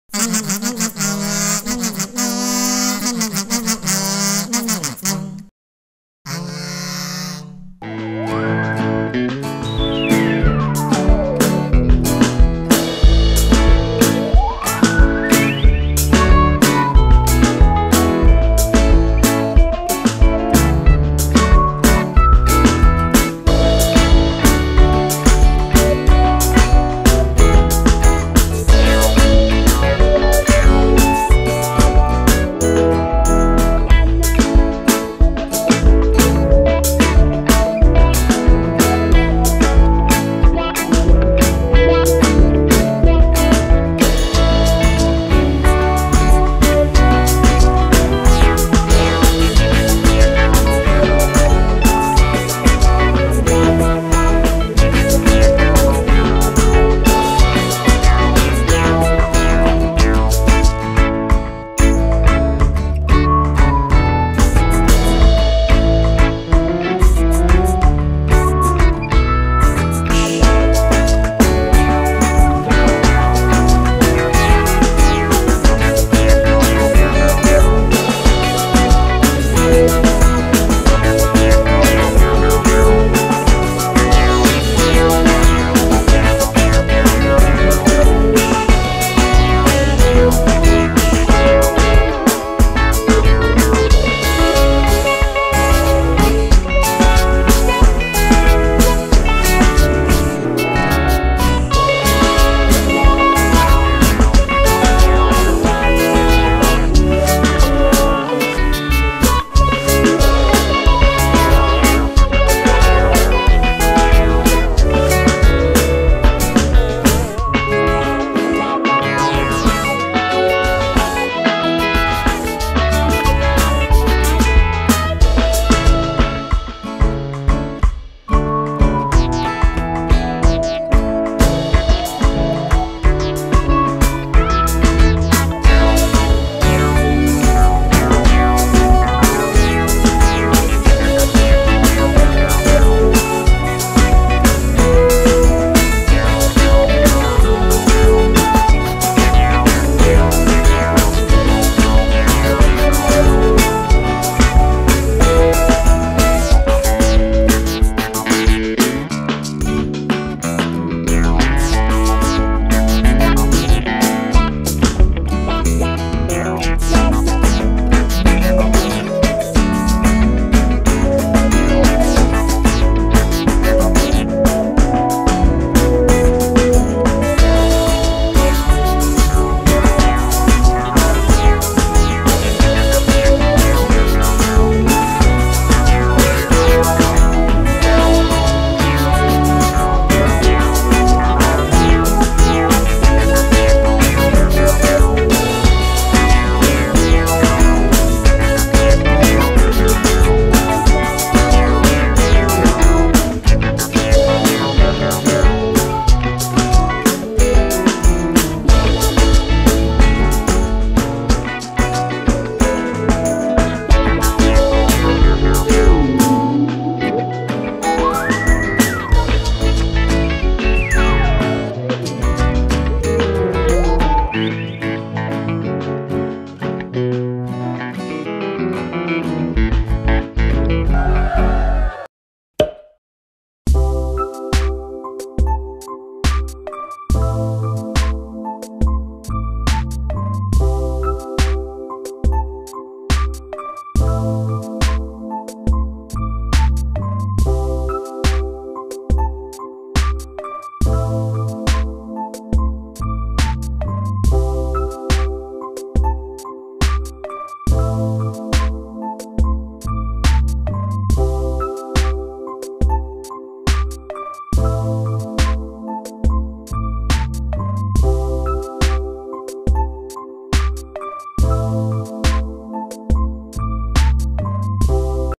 【彩繪聞森笛】瀏海剪壞的小萌貓 Omame ( cat kazoo)
人類如果剪壞瀏海會很崩潰...
但若是貓咪剪壞瀏海卻超級討喜阿～～～

推薦影片：
【彩繪聞森笛】